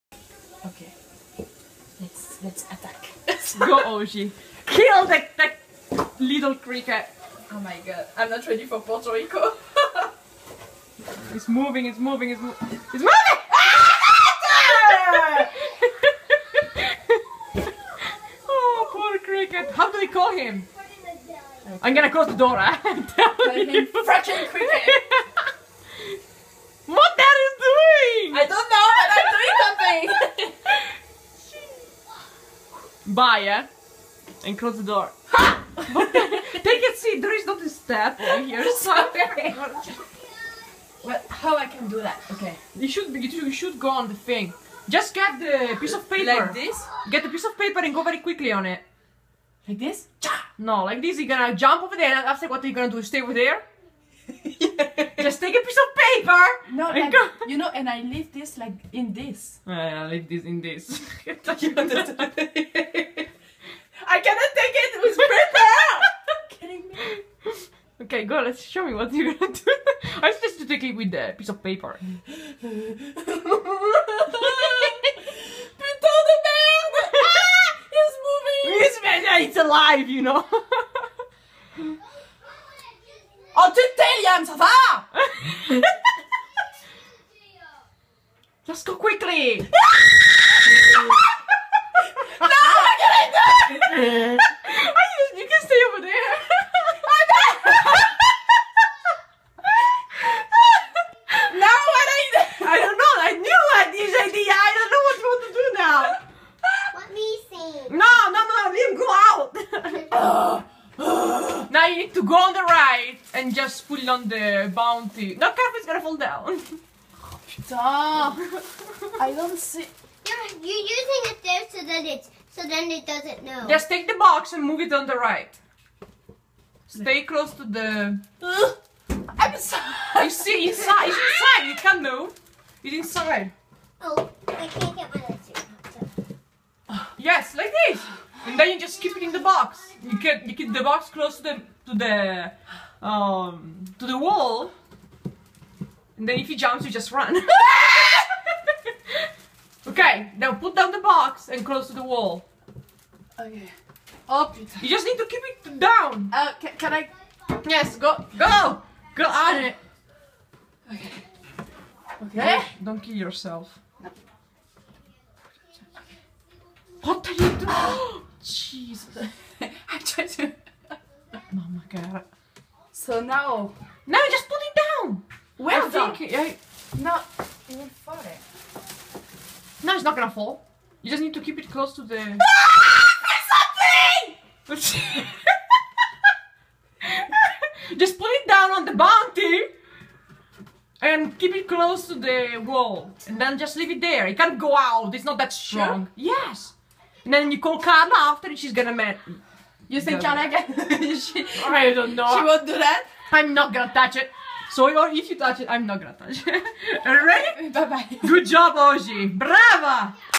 WHEN YOU FIND A FREAKING GIANT CRICKET IN YOUR BATHROOM AND YOU TRY TO CAPTURE IT 😱
My friend and I found this GIANT cricket in her bathroom, then she tried to capture it. She was afraid to find her new friend in her bed during the night. (I don't even know why bugs big like that exists in the world. )
PS the cricket is still there in that box...